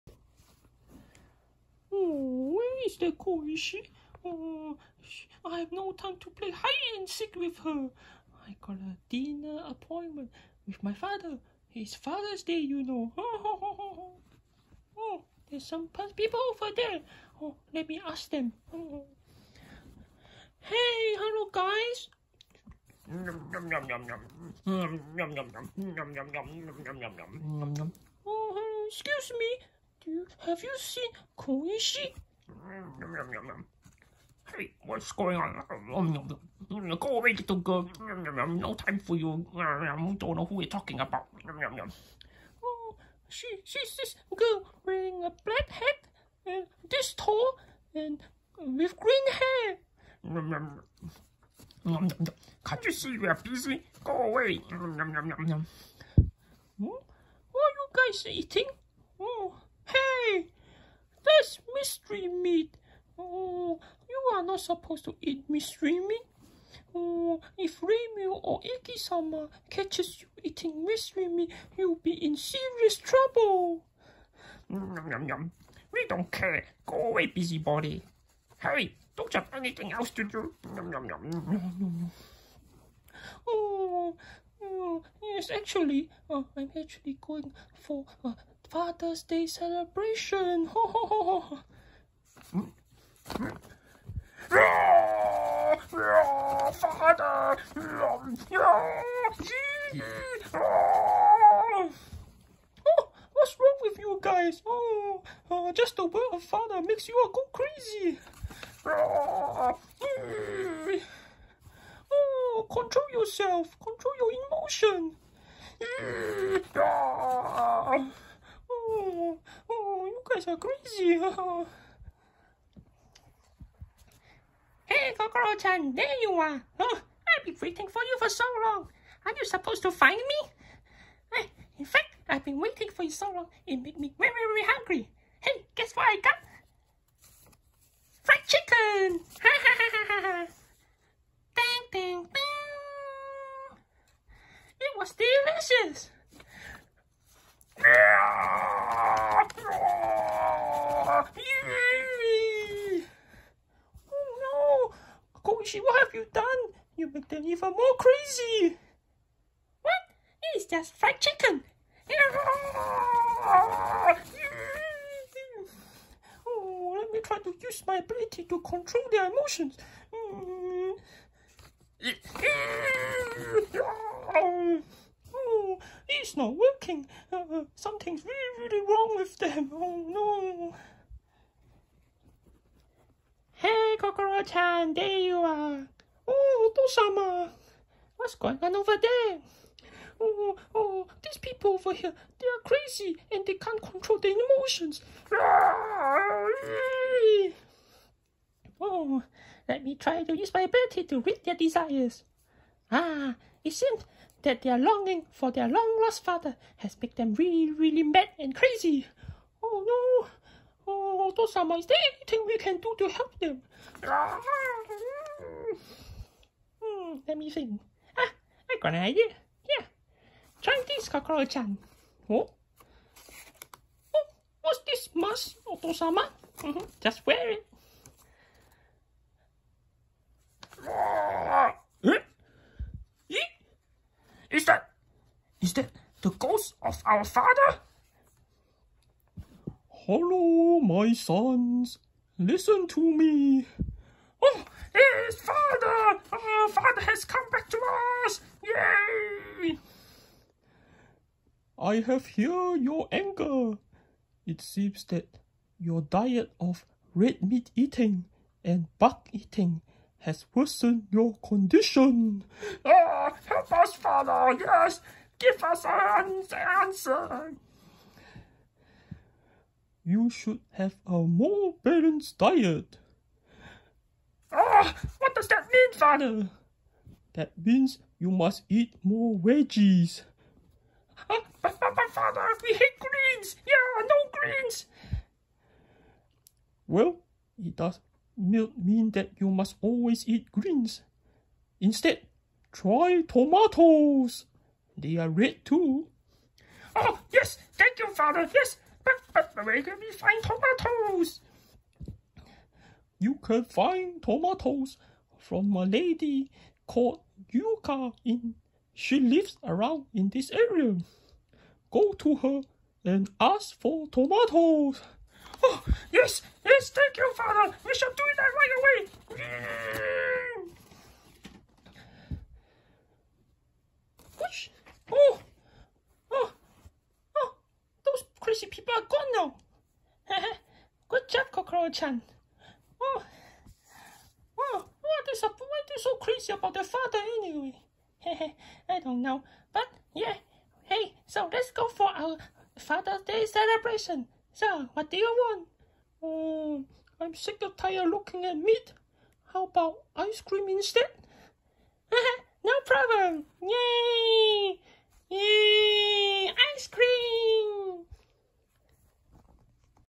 Battle Screwdriver 20K LOL Father’s Day Special / Kokoro bumps into the Red Feather Gang Touhou Fumo
Press S to spit on Sickguinius

Miko Fumo
Koishi Fumo